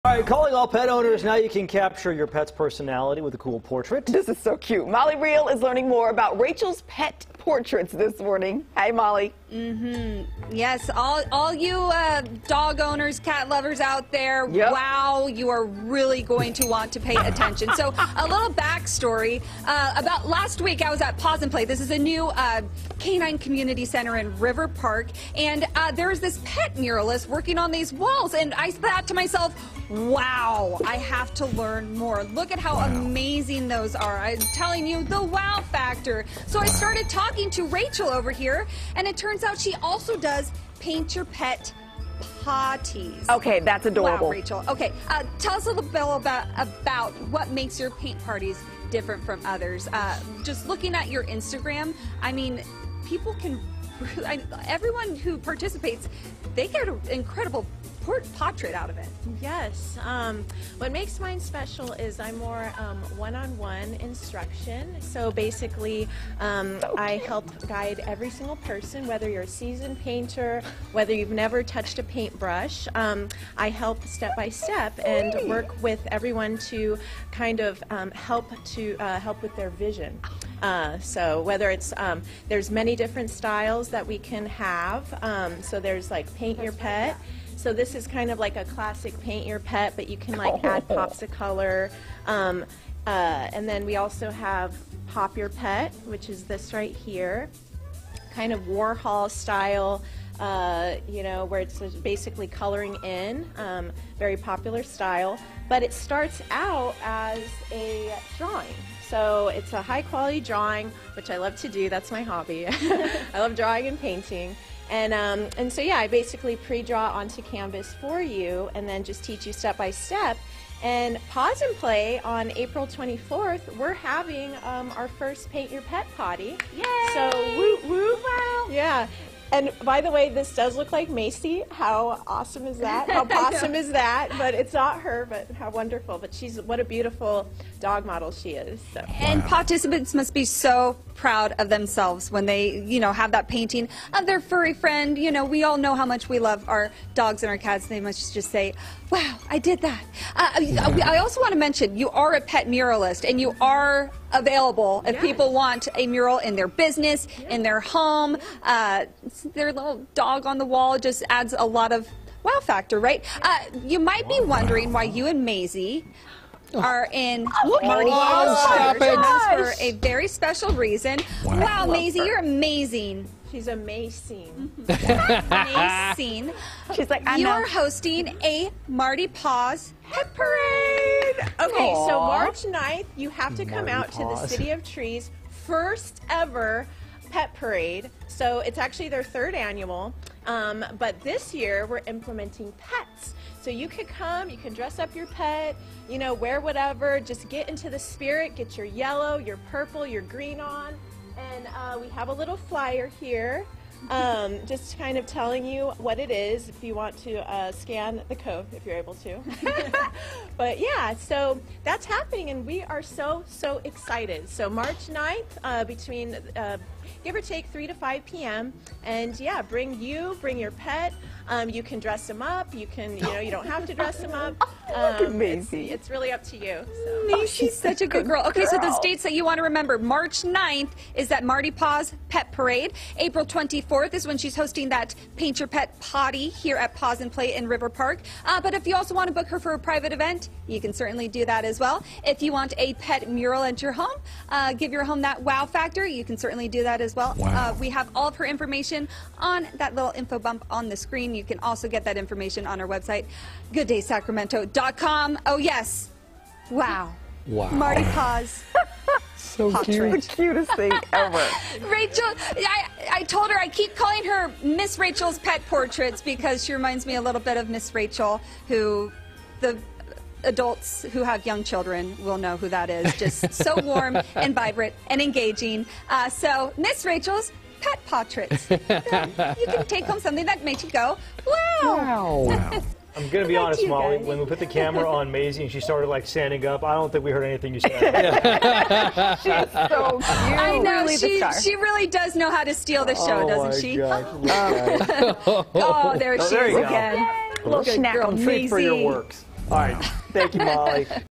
Rachel's Pet Portraits
Molly Riehl is in River Park at Rachel's Pet Portraits showing us how you can get painted portrait of your pet!  Check out Rachel's awesome artwork to honor your furry friend.